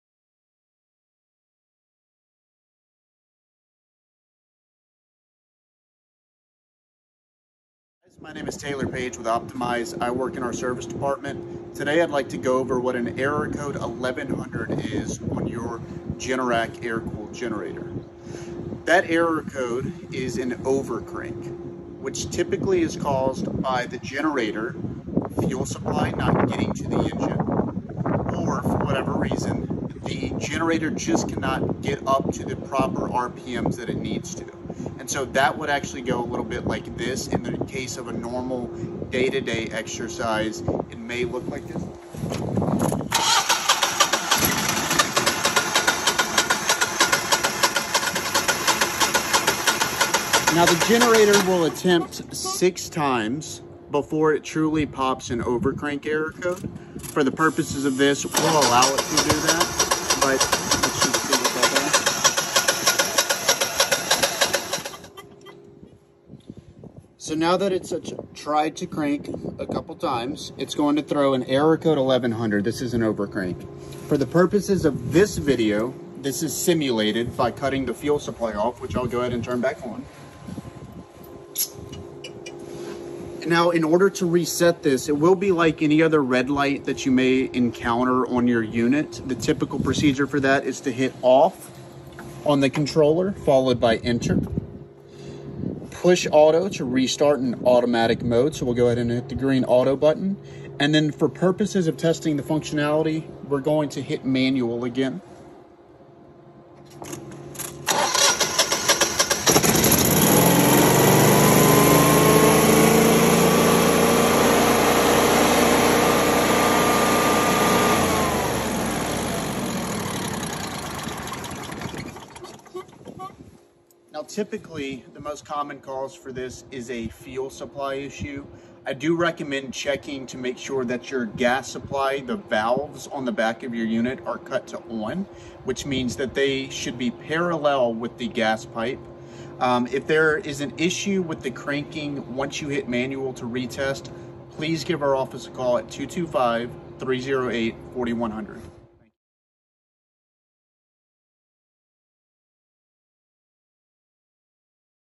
Error Code "1100" Instructional Video
This video explains what an "1100 Overcrank" error code is and common avoidable causes of the issue.